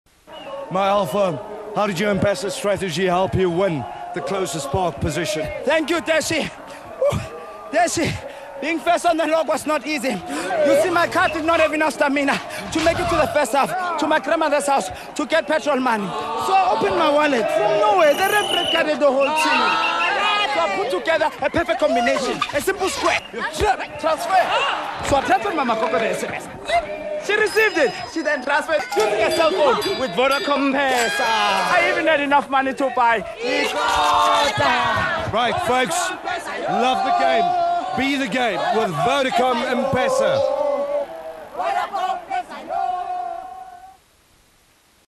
Vodacom - Mpesa
A very busy soccer themed advert.